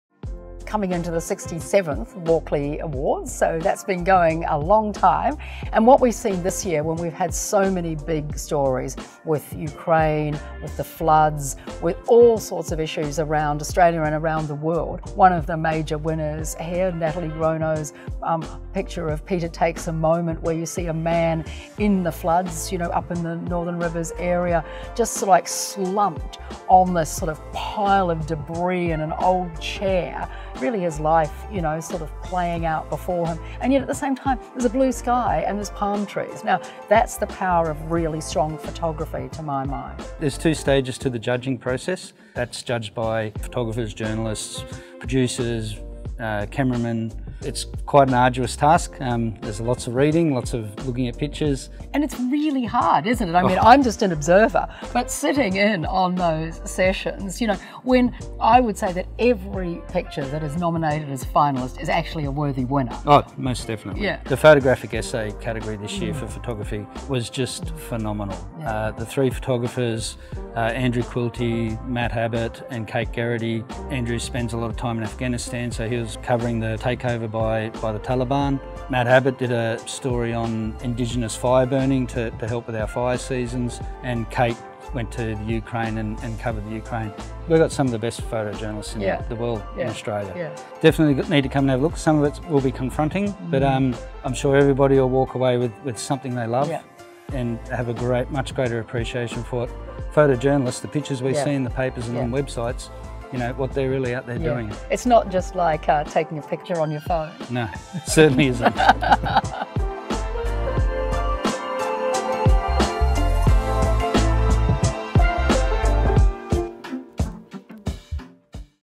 Exhibition - Walkley Awards for Excellence in Photojournalism @ NSW Parliament (Nov-Dec 2022)
Until 15 December 2022, the works of Australia's best photographers – as recognised by the Walkley Awards for Excellence in Photojournalism – are on display at NSW Parliament House.

Visit the Parliament on weekdays between 9am and 5pm at 6 Macquarie Street, Sydney. Entry is free. Learn more and preview the exhibition